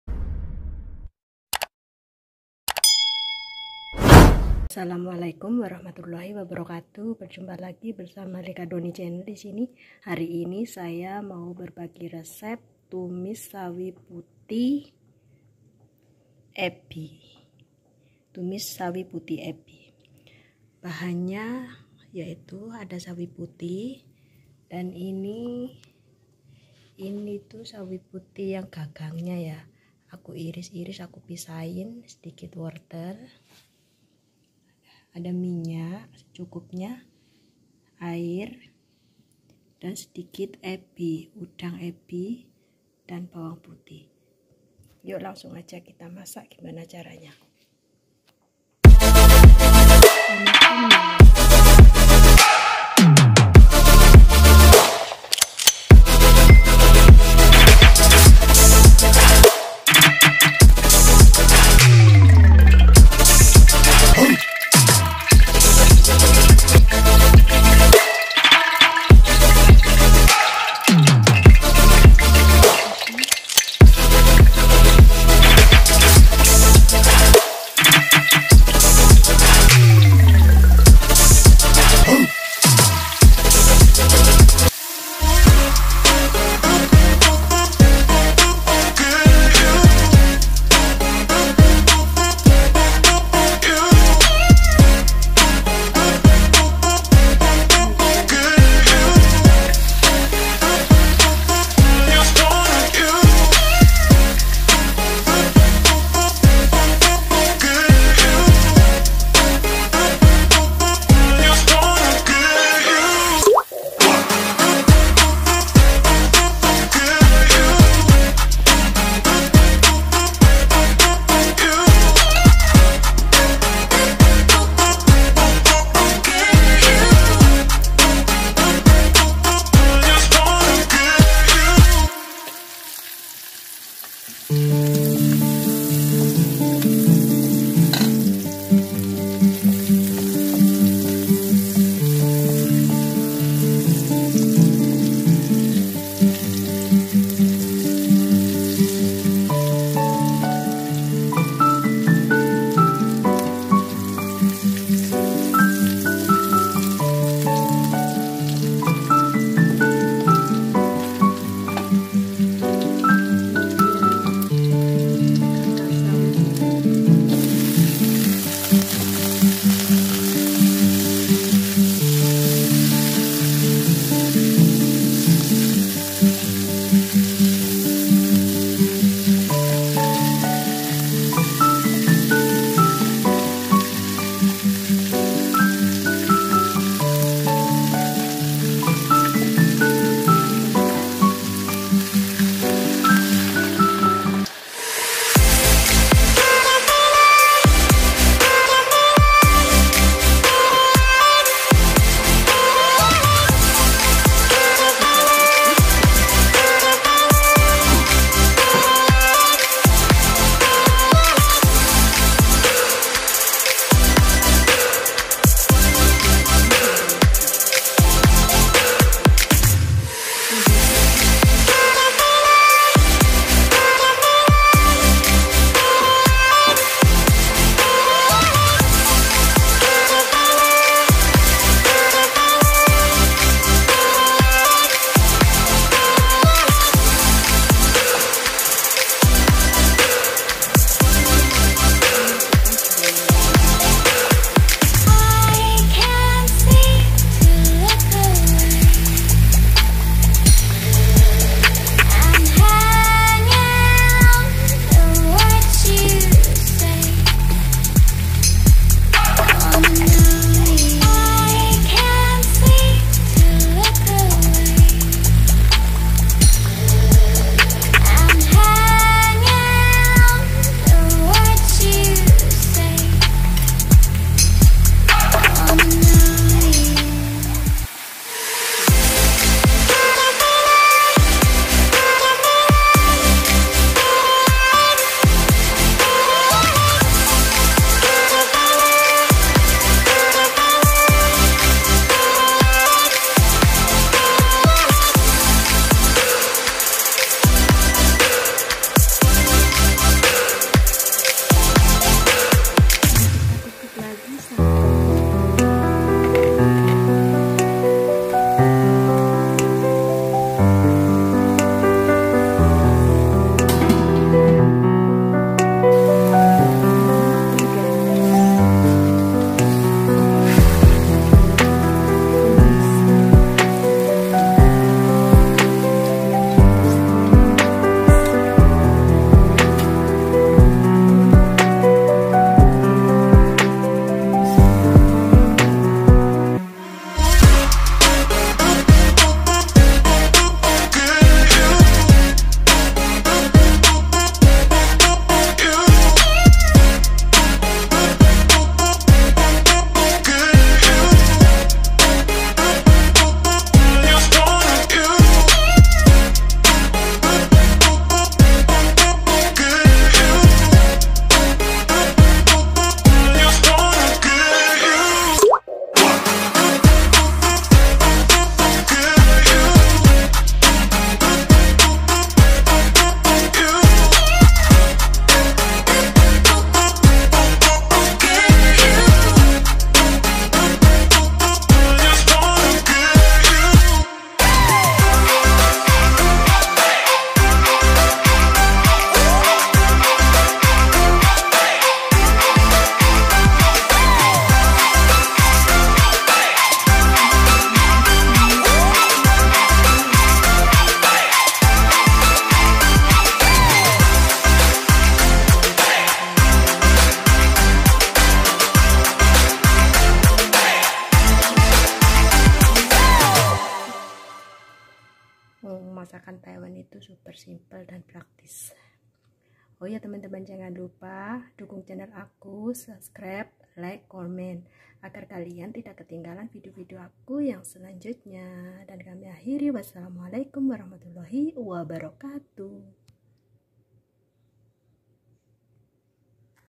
MENU RESEP TUMIS SAWI PUTIH UDANG EBI ALAH"TAIWAN
Hai teman "Berjumpa lgi  bersama likah  doni chanel disini
Hari  ini  saya  mau  beragi resep  cara tumis sawi putih udang ebi alah"taiwan